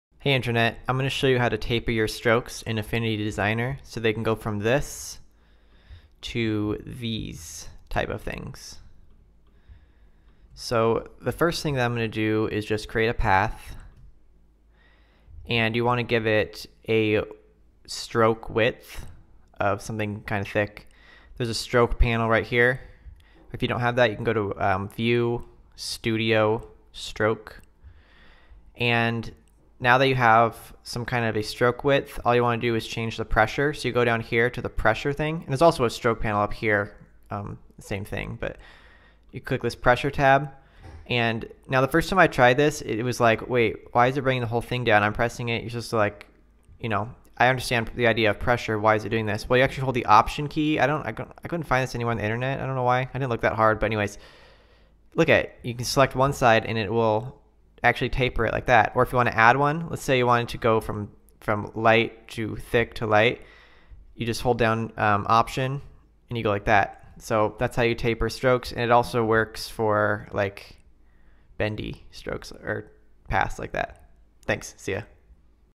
How to Taper Path Strokes in Affinity Designer
This is how to create a tapered stroke on a path in Affinity Designer.

You use the "Pressure" setting in the Stroke Studio. You have to press "option" on Mac for the node to actually go down individually, rather than bring both nodes down.